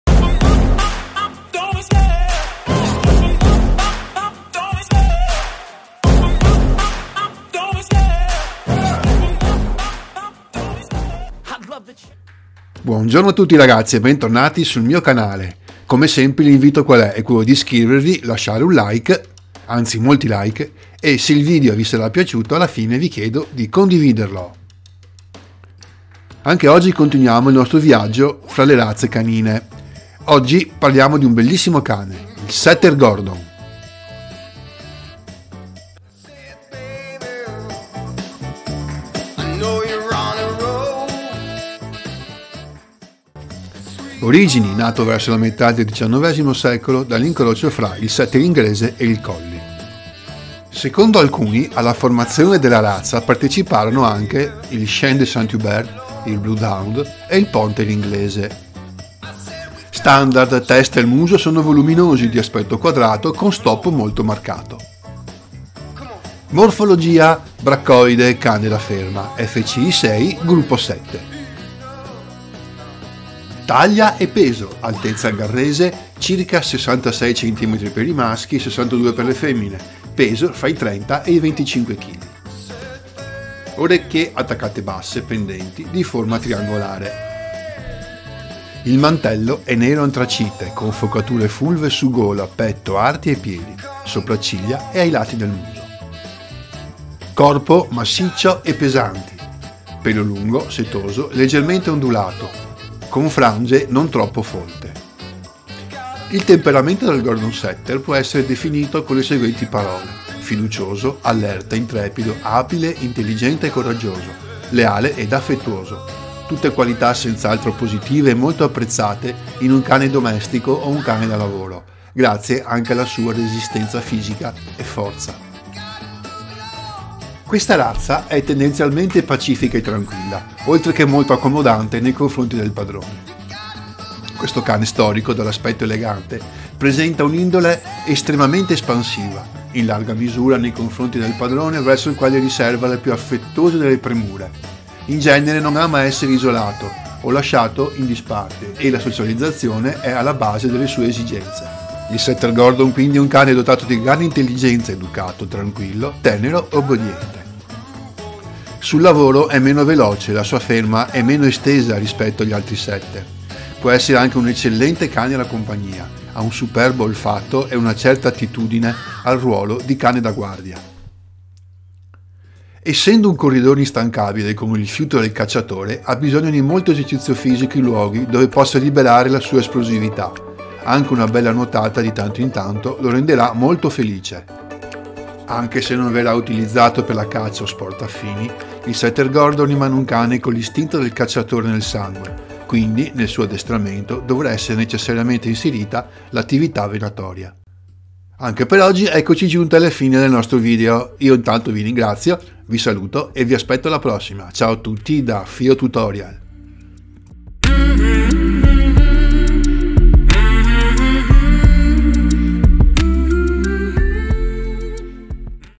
Il Gordon Setter
Il Gordon Setter. Meravigliosa razza , buona, tranquilla...ma con la caccia nel sangue